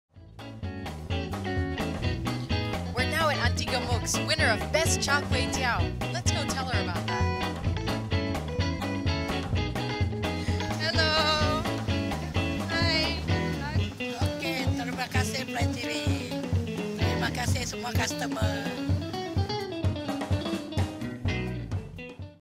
Best Char Kuey Teow 2010 | Aunty Gemok FAM
The LOVE your Street Food campaign and Annual Foodsters' Awards present you the 2010 champions